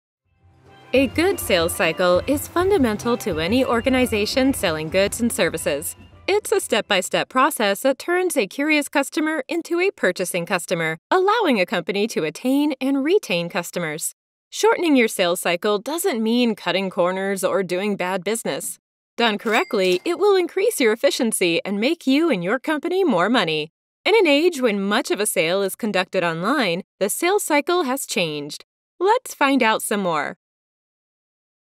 Shortening your Sales Cycle - Course Trailer - TalentLibrary™
Train your sales managers on the importance of the Sales Cycle in business and show them how to shorten the sales cycle with this course.

Create your free account, get access in a few clicks, and start training right away

What's covered in this course:
--The importance of the Sales Cycle in business
--How a shorter Sales Cycle suits a digital age
--How to  shorten your Sales Cycle

Why your teams need this course:
No matter what you sell, this course can help you increase your efficiency. This course will help your sales managers get the most out of their own time and that of their sales team and will put them on the path to boosting both confidence and sales.